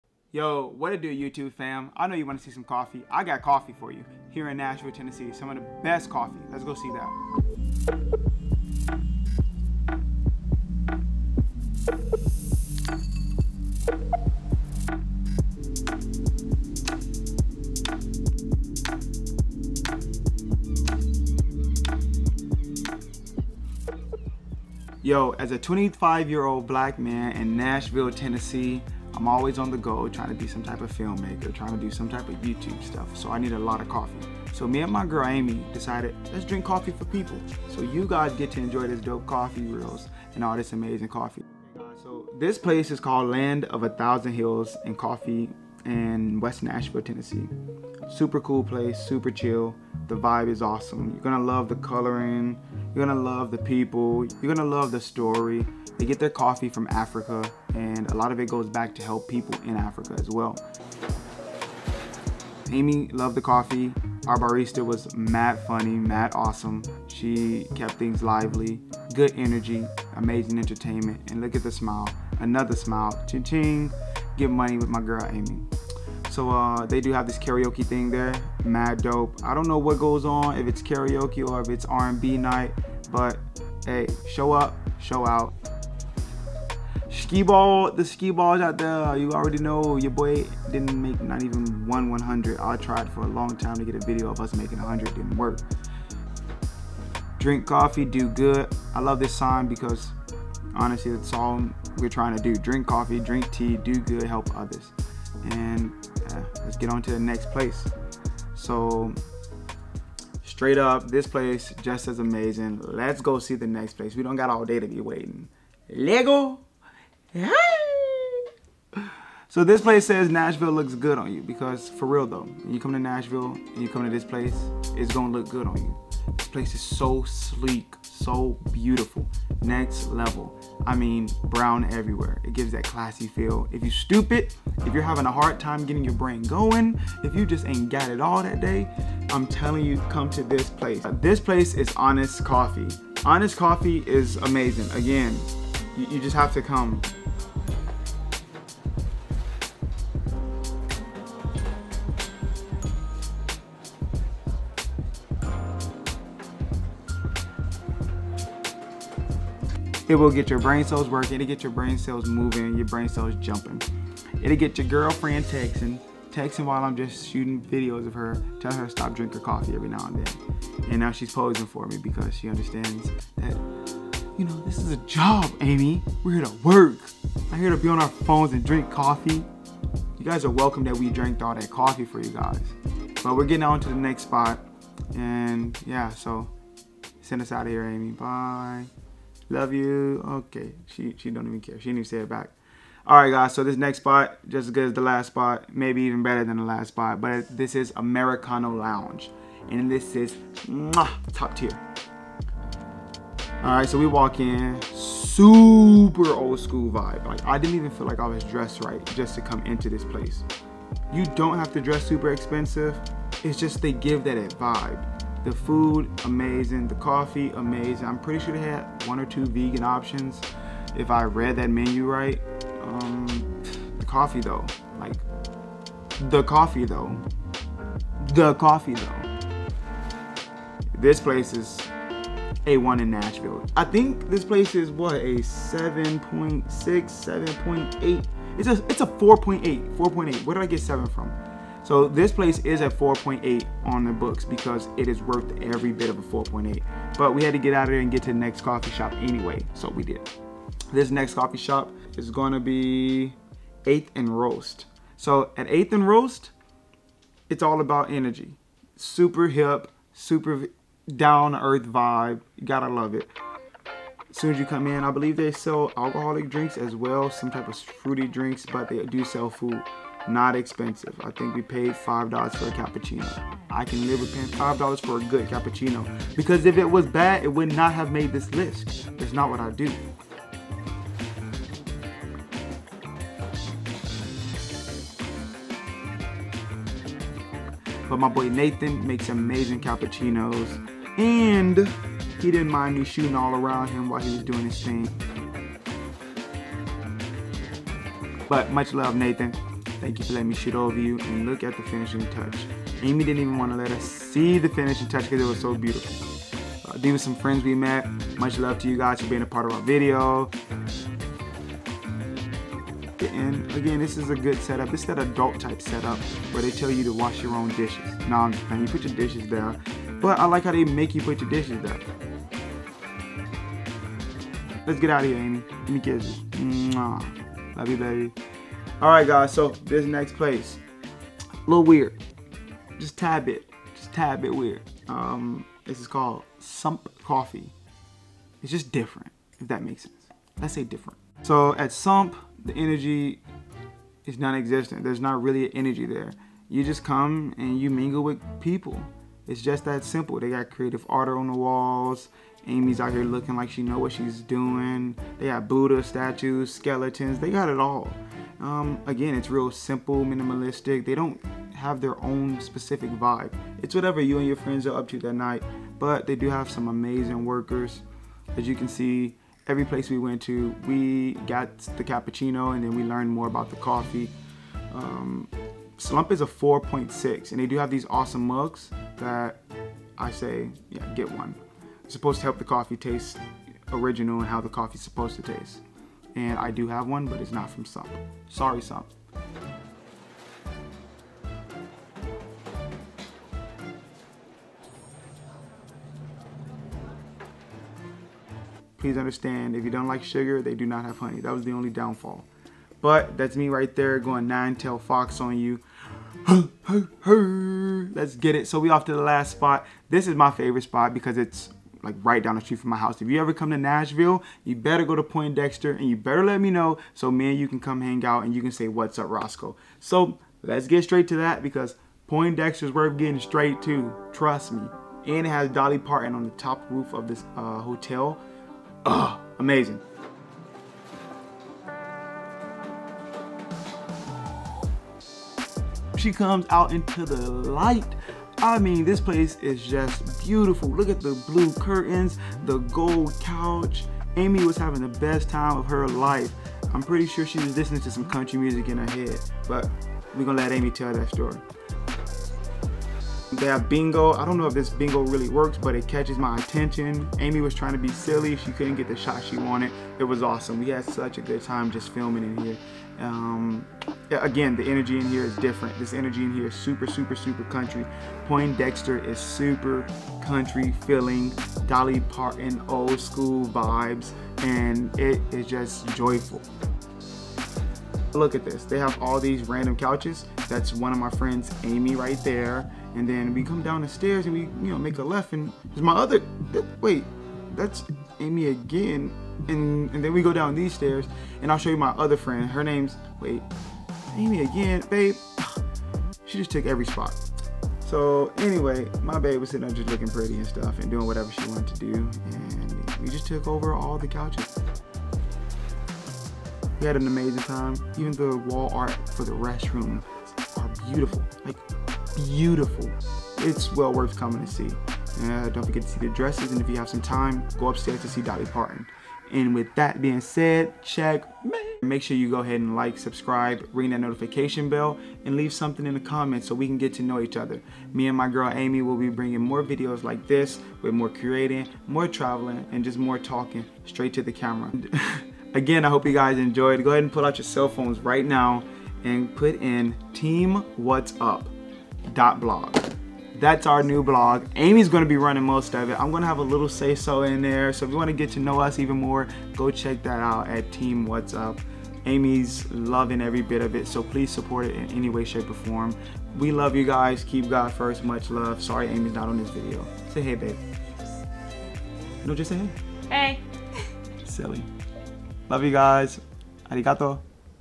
Nashville, TN | Charming Coffee Spots For Your Next Trip To Nashville.
These are the best coffee Spots in Nashville. As a Local and Digital Creator here in beautiful Nashville Tennessee, I know all the best places to get Coffee and also Amazing Content for Social Media. Come along with me and my fiance ( Amy Jo ) as we explore the BEST of the BEST.

Much love, GOD Bless, and Enjoy!